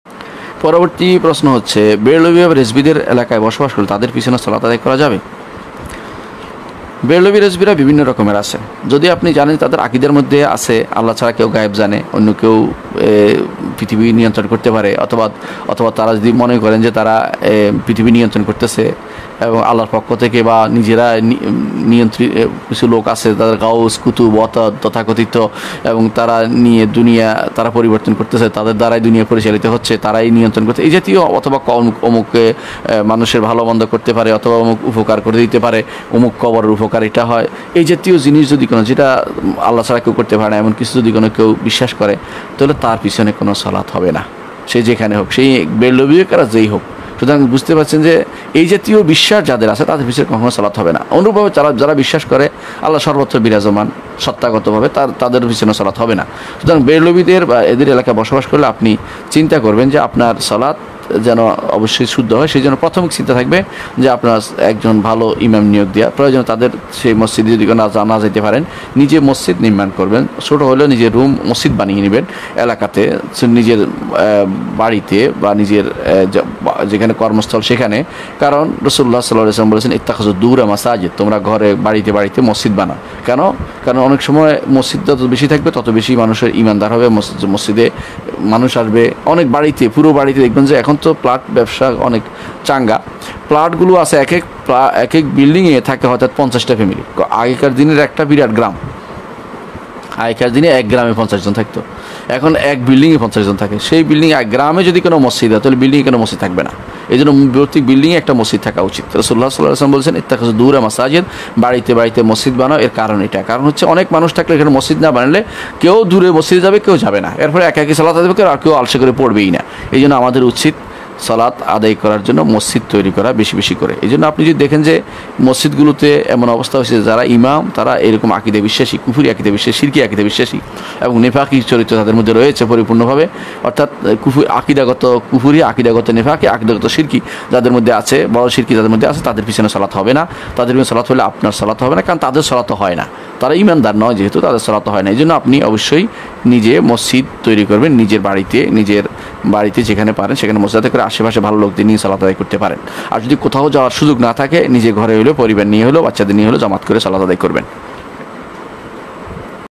বেরলভী ও রেজবীর পিছনে নামায হবে কি? | শাইখ আবু বকর মোহাম্মাদ যাকারিয়া | Abu Bakar Muhammad Zakaria
বেরলবী ও রেজবীর পিছনে নামায হবে কি? | শাইখ আবু বকর মোহাম্মাদ যাকারিয়া | Abu Bakar Muhammad Zakaria

নিম্মের plalist থেকে পছন্দের শায়েখ গনের ওয়াজ ও প্রচার কবুন